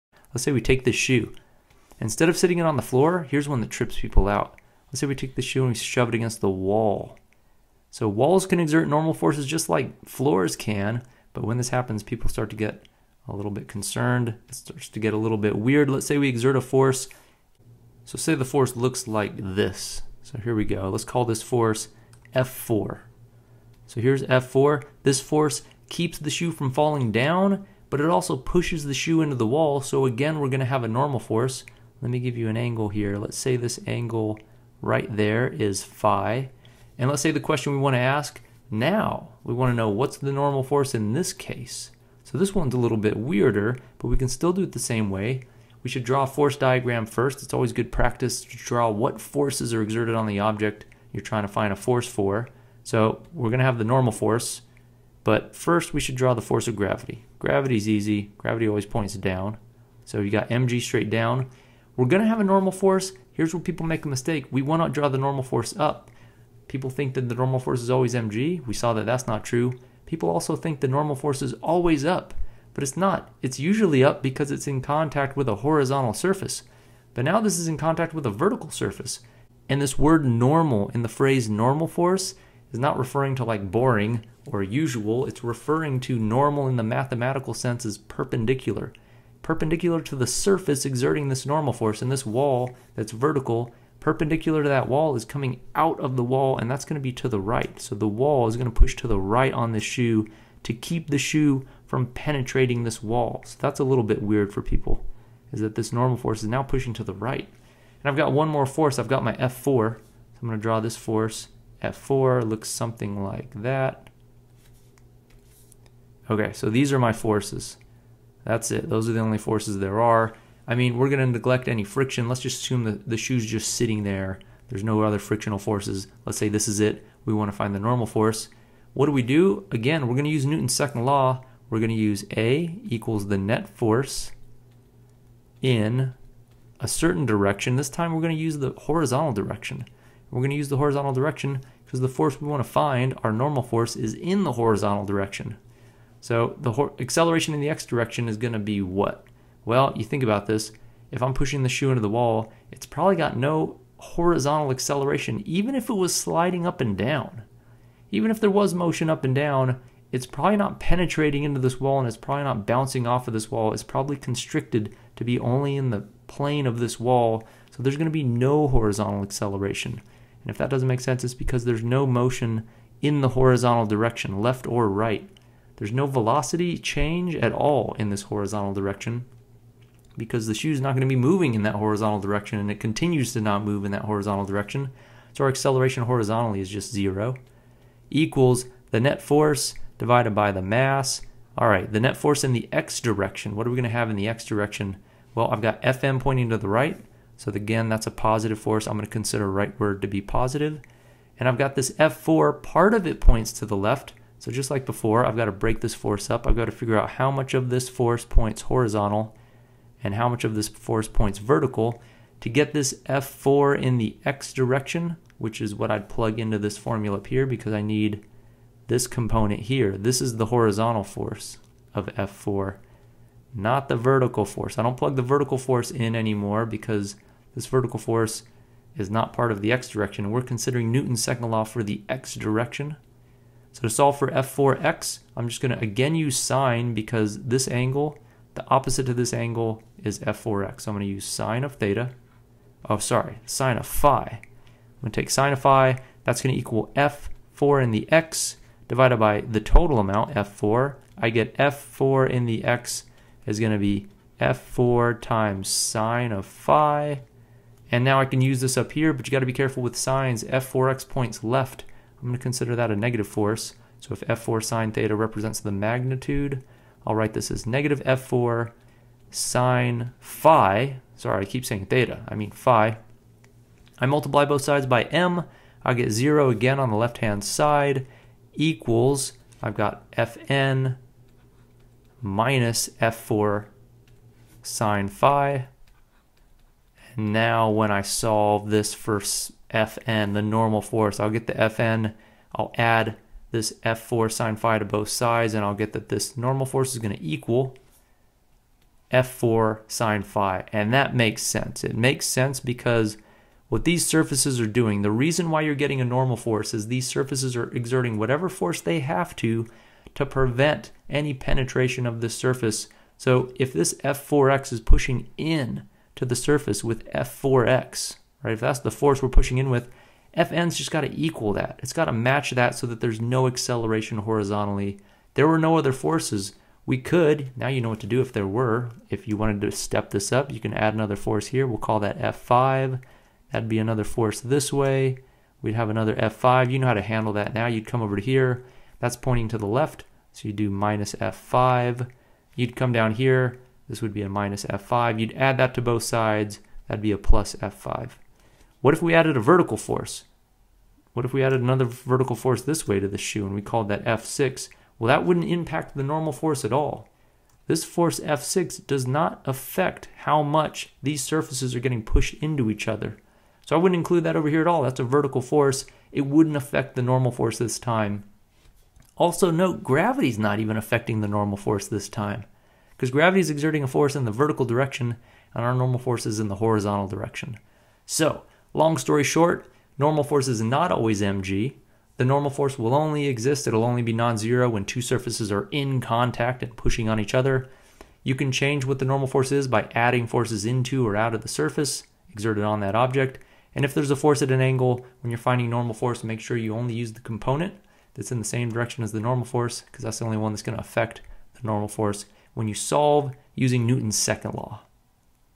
More on Normal force (shoe on wall) | Physics | Khan Academy
David shows how to determine the normal force for a shoe shoved against a wall with a diagonal force.